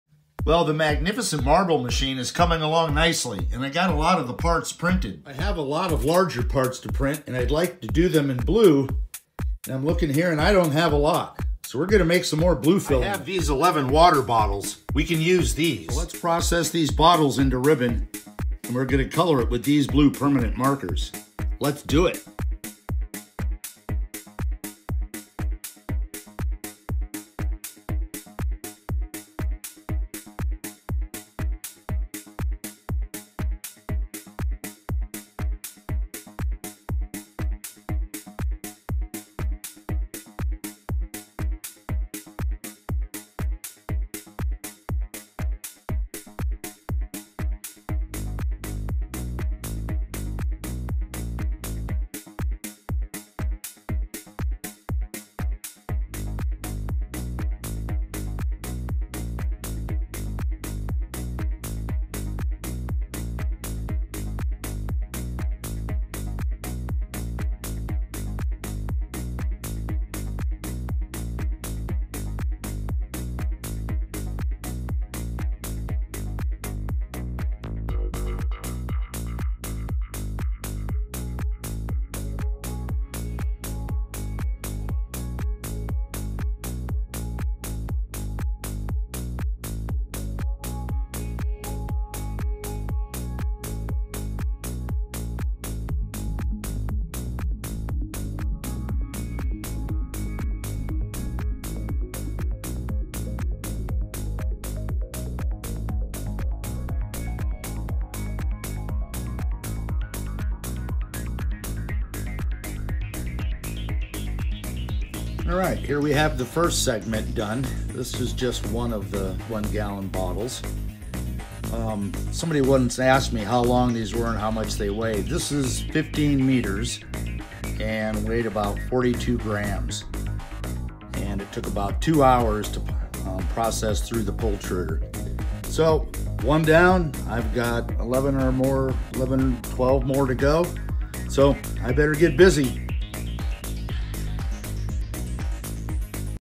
Making some FREE Blue PET 3D Filament from recycled 1 Gal water bottles! Using the Pultruder!
Using some 1 gallon PET plastic water bottles and some blue permanent markers to make more FREE filament for the Magnificent Marble Machine that I’m 3D printing!